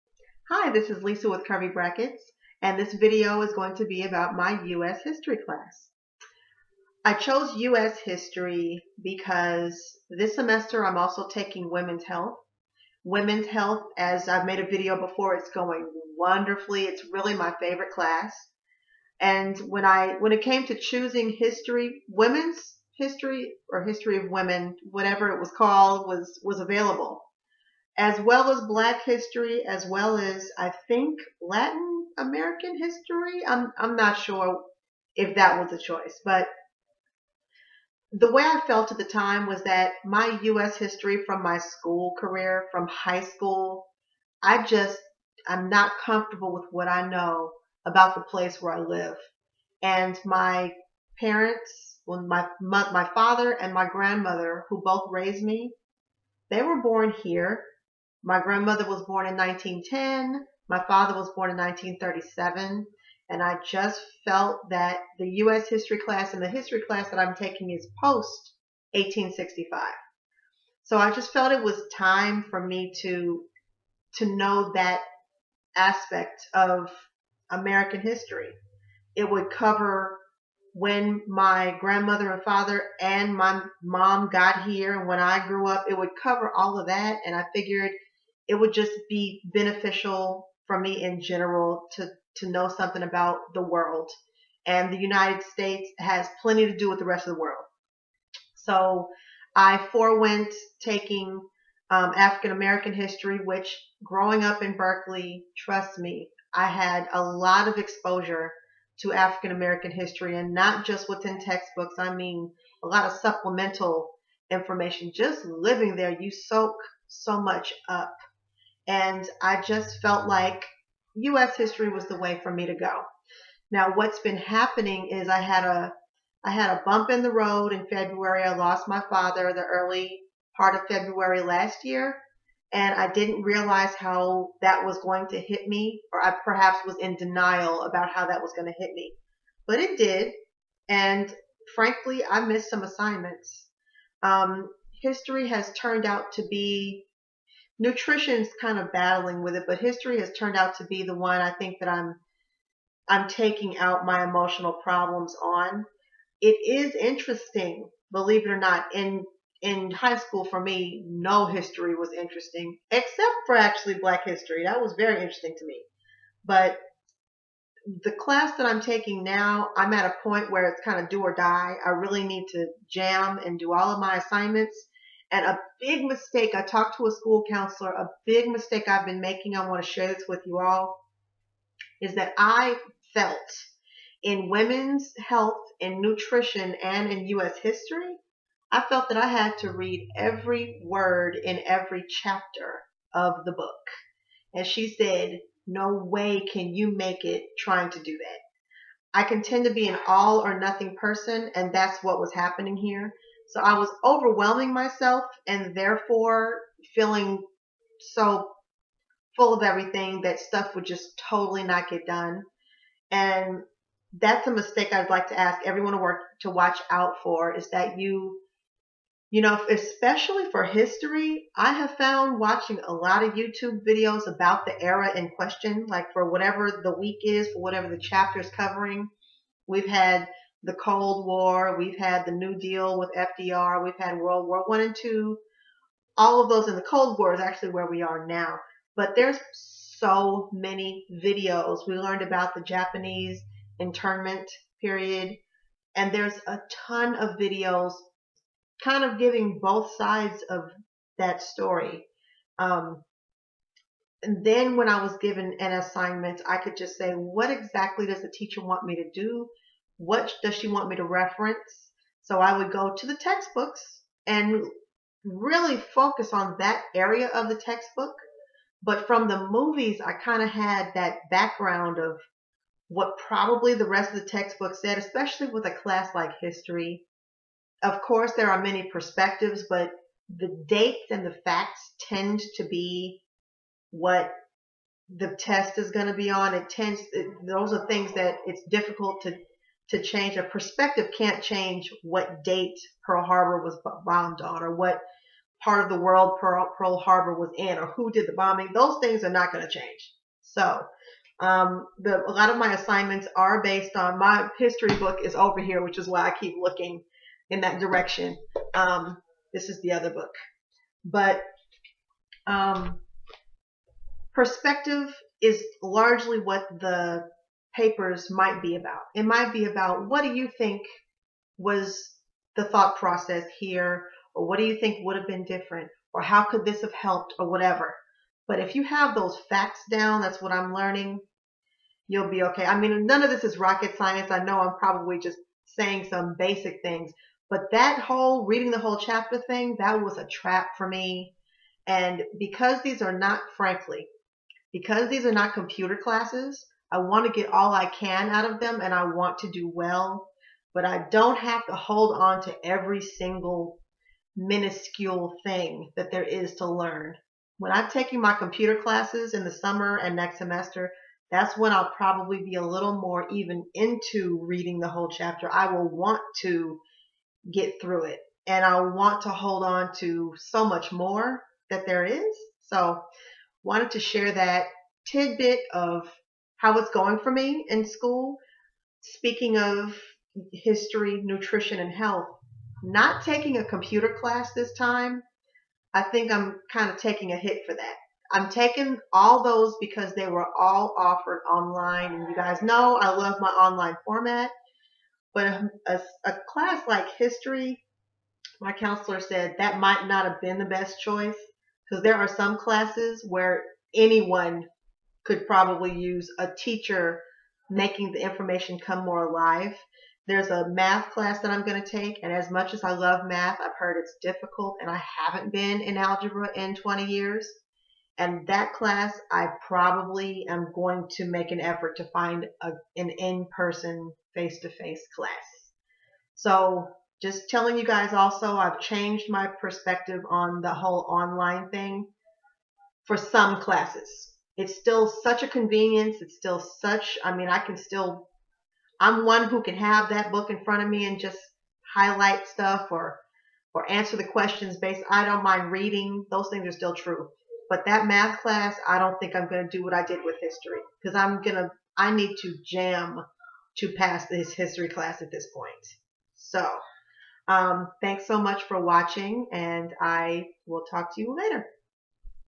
Is taking classes online for me after all?
My US history class has been quite a journey.  But how has it gone taking it online?

Filmed 3-31-15